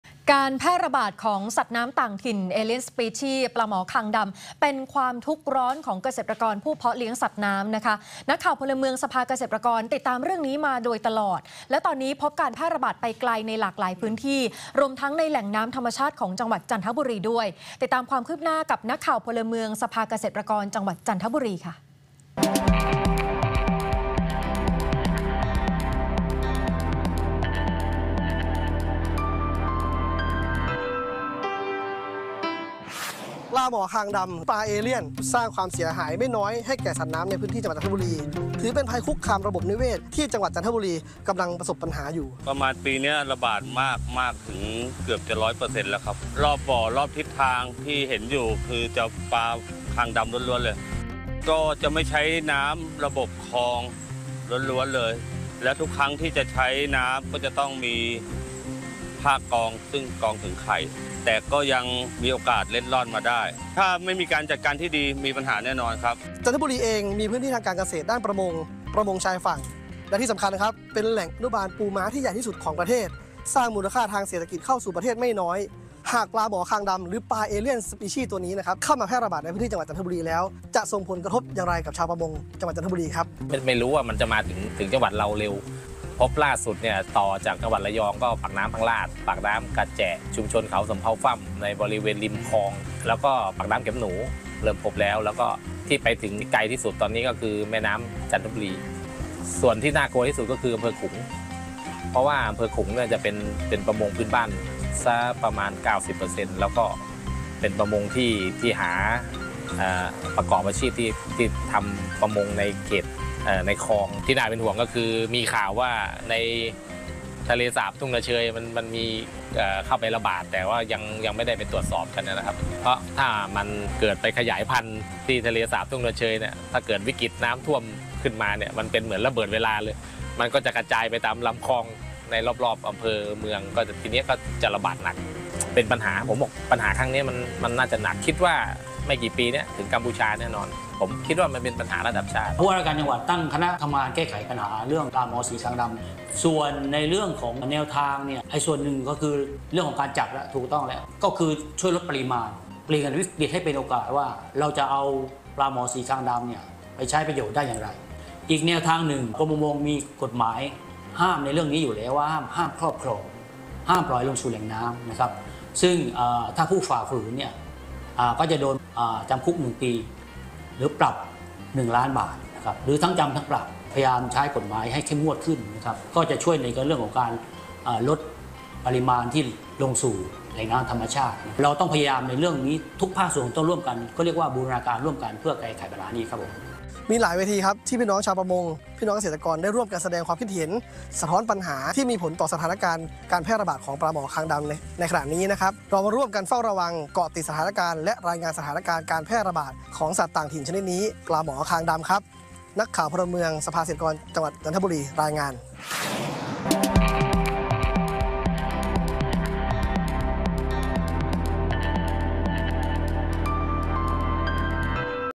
นักข่าวพลเมือง: ปลาหมอคางดำเอเลียนส์สปีชีส์ บุกแหล่งน้ำจันทบุรี จ.จันทบุรี 10-12-66 / 19.00 น.
นักข่าวพลเมือง การแพร่ระบาดของสัตว์น้ำต่างถิ่น เอเลียนสปีชีส์ ‘ปลาหมอคางดำ’ เป็นความทุกข์ร้อนของเกษตรกรผู้เพาะเลี้ยงสัตว์น้ำ นักข่าวพลเมืองสภาเกษตรกรติดตามเรื่องนี้มาอย่างต่อเนื่อง และตอนนี้พบการแพร่ระบาดไปไกลในหลายพื้นที่ รวมทั้งในแหล่งน้ำธรรมชาติของจังหวัดจันทบุรี ติดตามความคืบหน้าจากนักข่าวพลเมืองสภาเกษตรกรจังหวัดจันทบุรี

ในข่าวค่ำมิติใหม่ทั่วไทย ThaiPBS วันที่ 10 ธ.ค. 2566 เวลา 19.00 น.